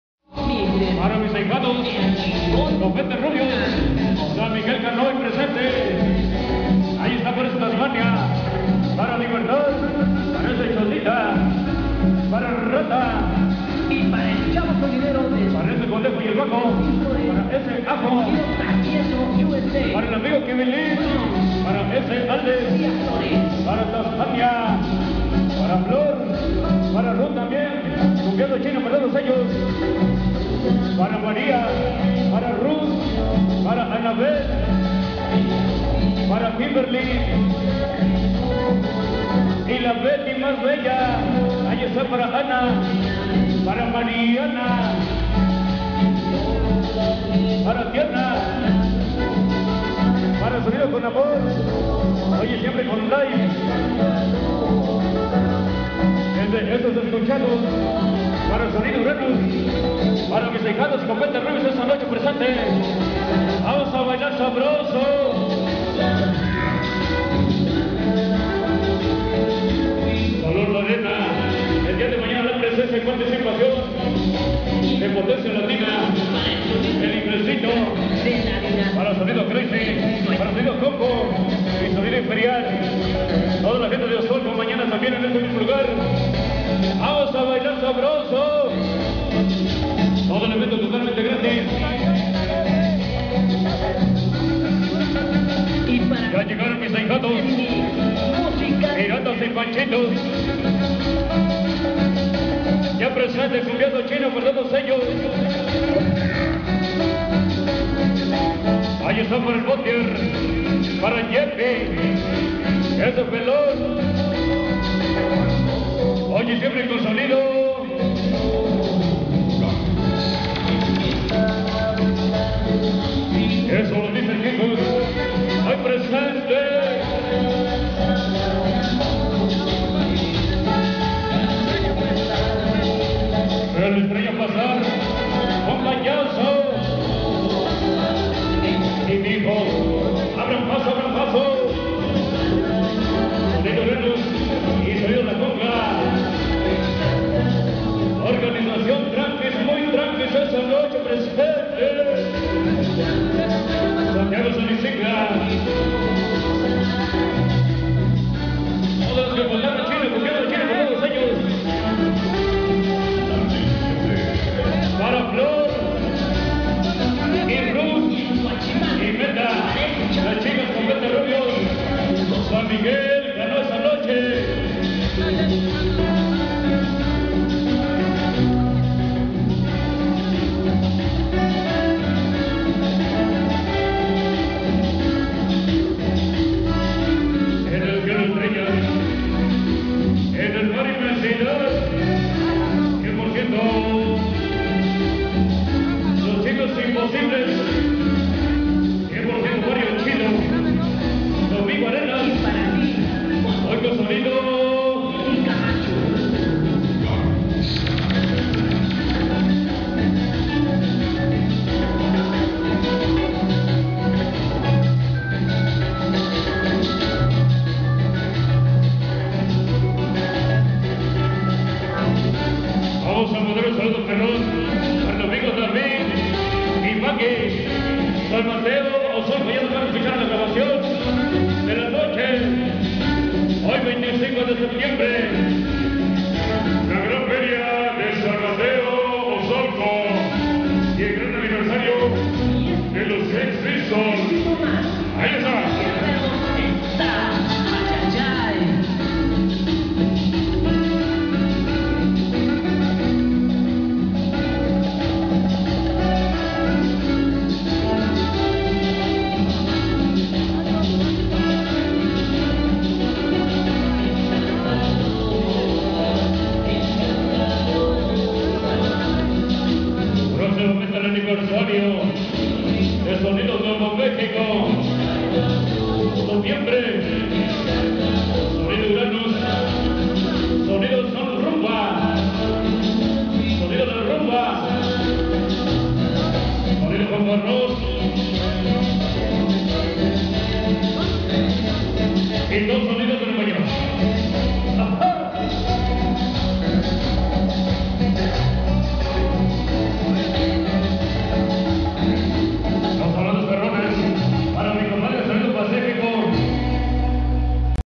!!!JOEL MORALES Y DJ CHINO !!SONIDO URANUS !!LIVE SALON LA ARENA!!
BUENO ESTE VIDEO ES PARA MIS COMPADRES SONIDO URANUS Y TODO EL STAFF TECNICO ,,DEPARTE DESU SERVIDO EDUARDO NOLASCO INTERNACIONAL NUEVO MEXICO ESTA FUE EL DOMINGO EN EL ANIVERSARIO DELOS EXPISTO Y LOS DUCKS ,,TODA LA BANDA SAN MATEO OZOLCO ,,,ECHALE CON SABOR!